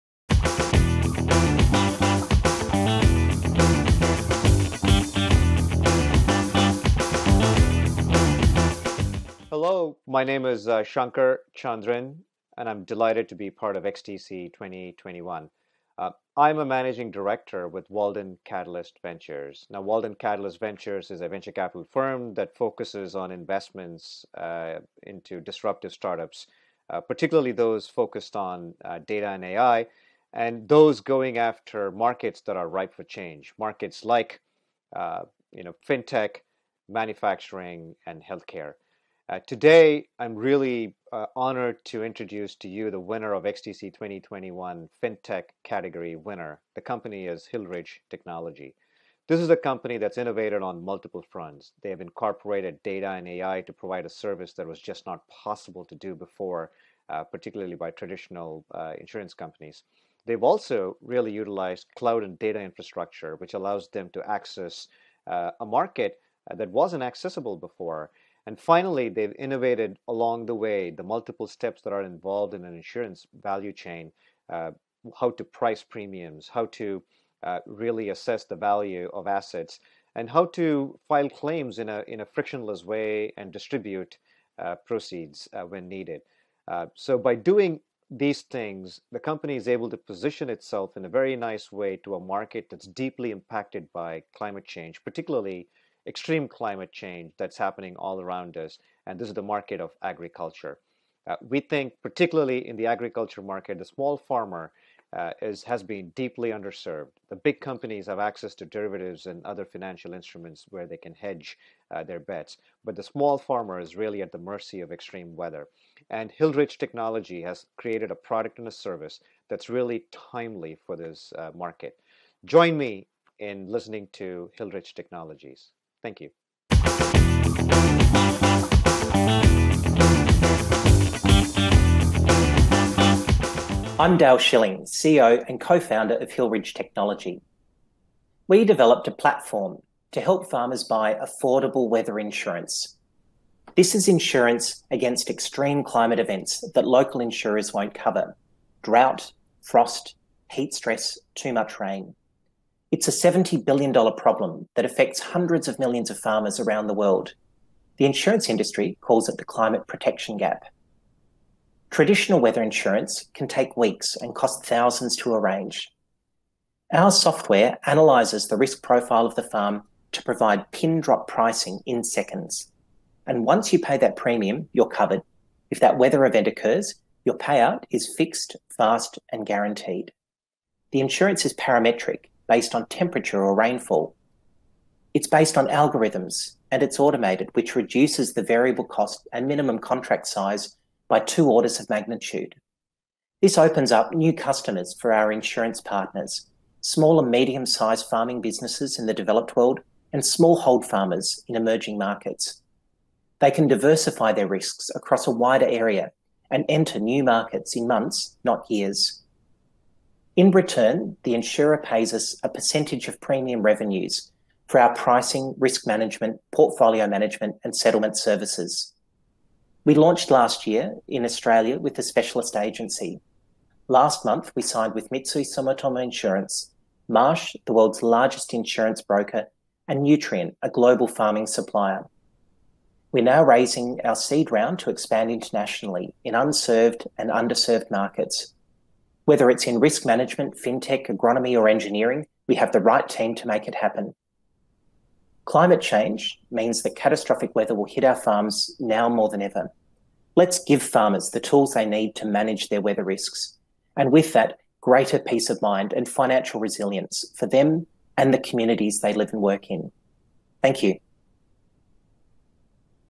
XTC 2021: Global Finals Pitch - Hillridge Technology
Fintech Winner: Hillridge Technology has developed weather-based parametric insurance for farmers to help protect crop yields and livestock.